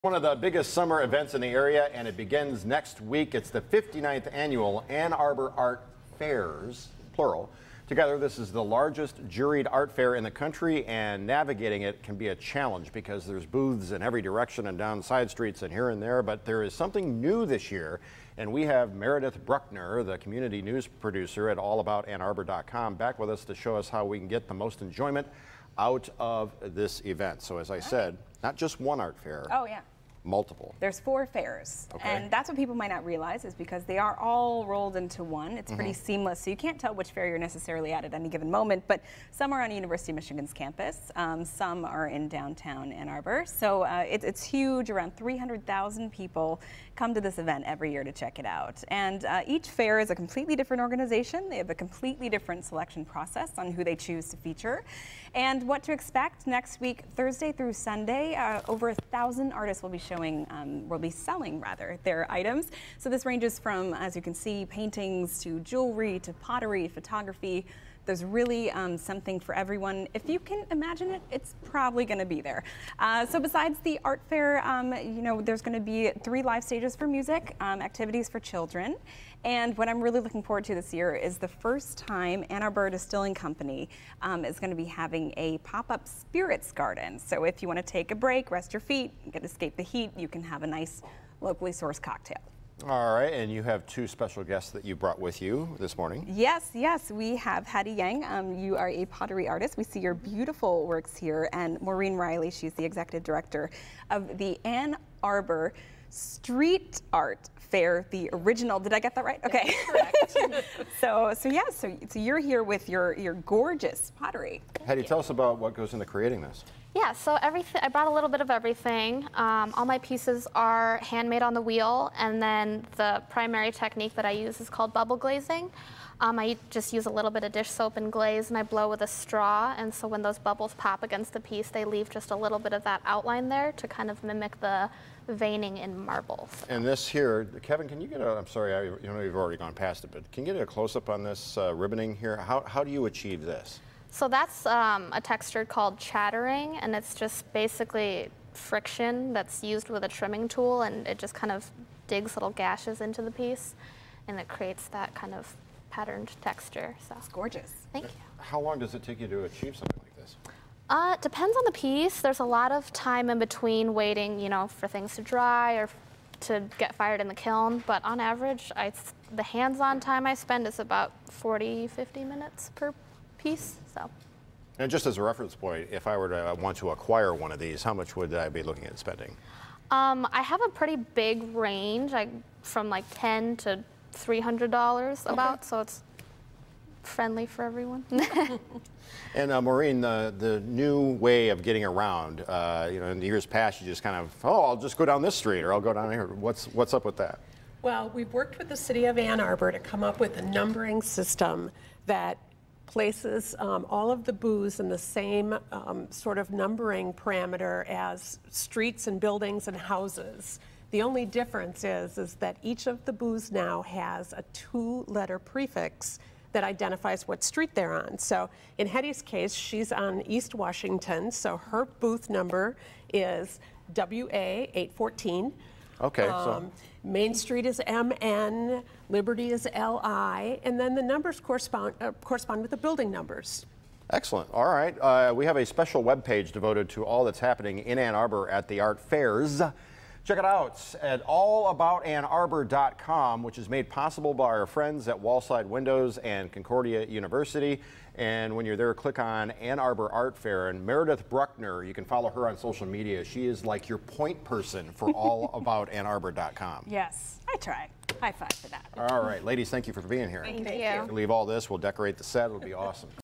Live in the D: Make your plans for Ann Arbor art fairs
The new way to help navigate one of the biggest summer events every year